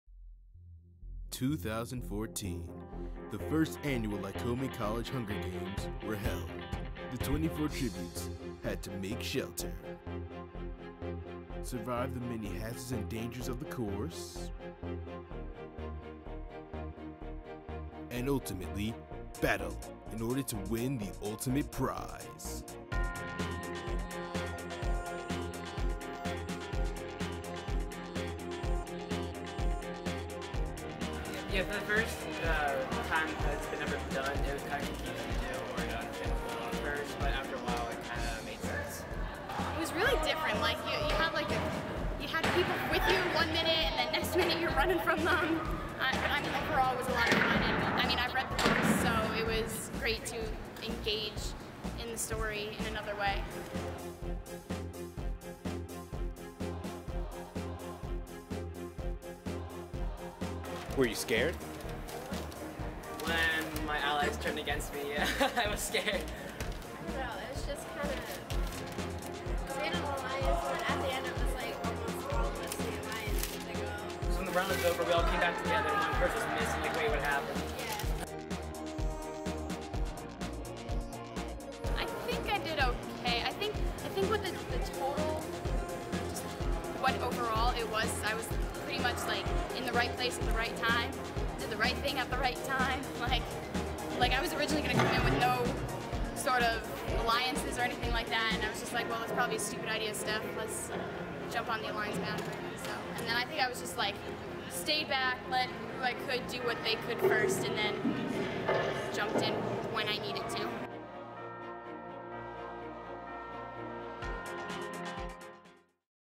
Lycoming College Hunger Games 2014
This video is a re-cap of the first annual Lycoming Hunger Games. The video was shot and edited by Robert Christian.